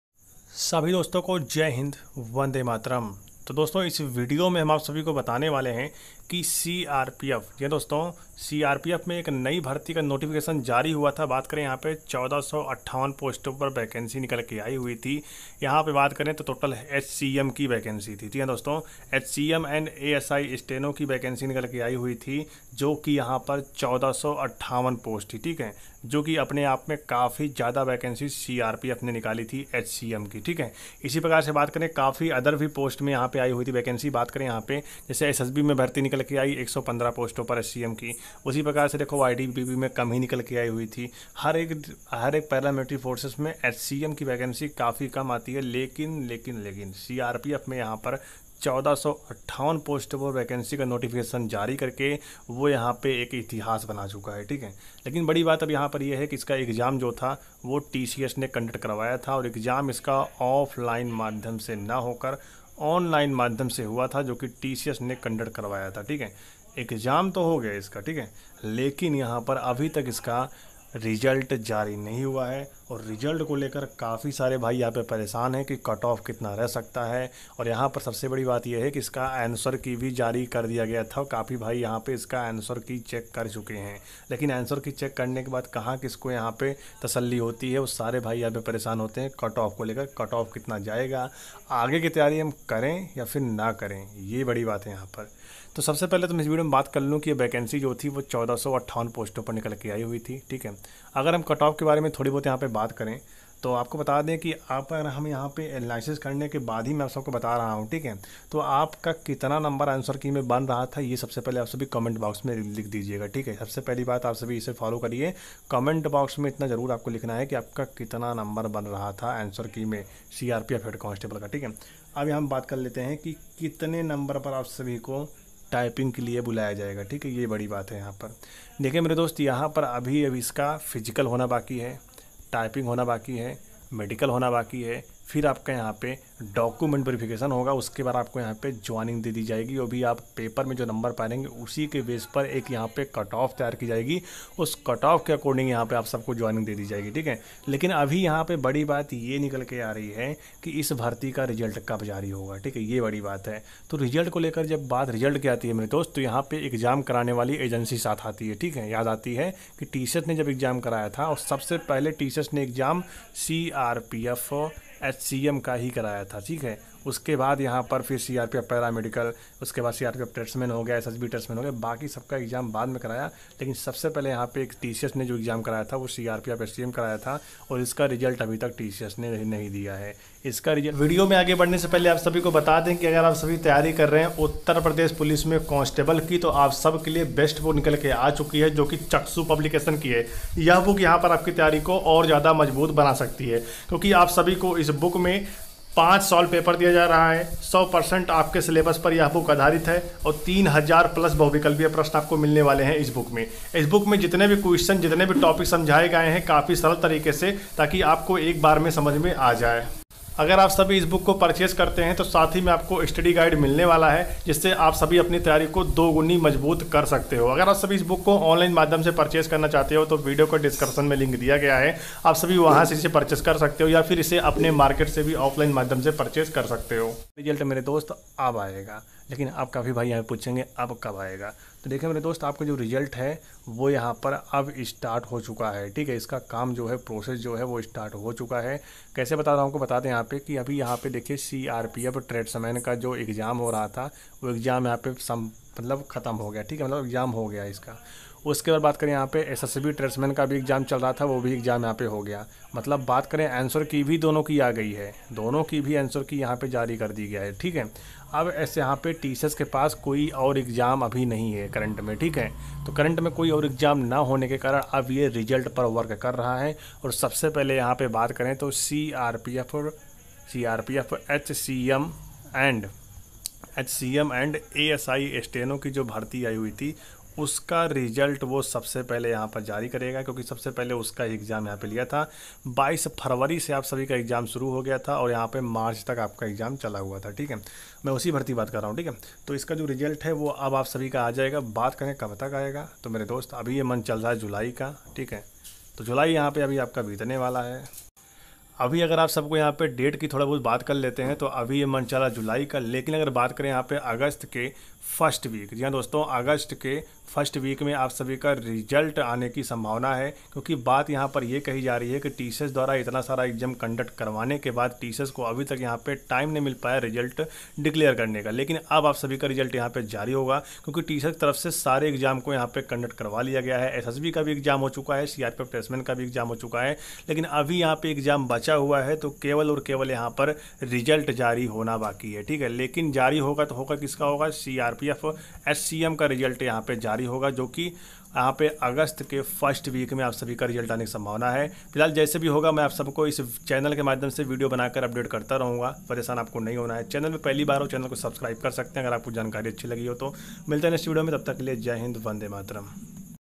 CRPF Head Constable Ministerial 2023/ Result Date Out Cut off बहुत कम जाएगा Today Update
DESCRIPTION FOR UP POLICE CONSTABLE BOOKS  2023